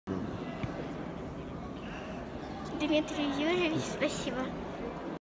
Дмитрию Юрьевичу Котвицкому большое спасибо от Паниной Дарины
1 место на Чемпионате Области. Дарина Алексеевна. Сколько нужно человеку для счастья ?! Почувствовать что ты можешь. Что ты не хуже других. Что к твоему мнению прислушиваются. Что ты что то значишь и от тебя что то зависит

1 место на Чемпионате Области. Дарина Алексеевна